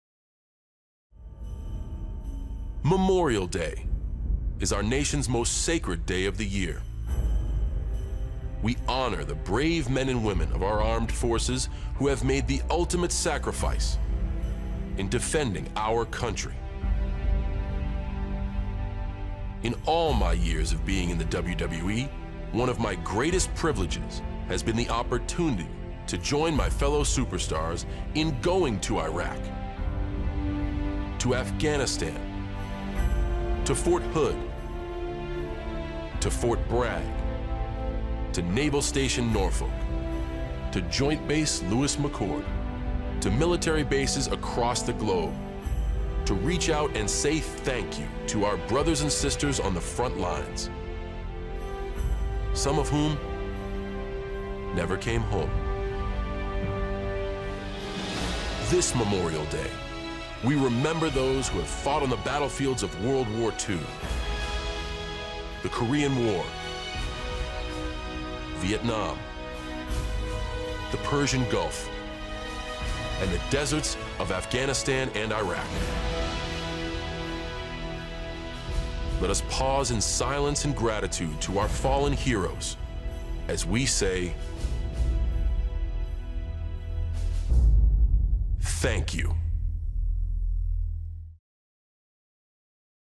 John Cena and WWE honor our fallen heroes
John Cena honors those who have fallen when serving our country.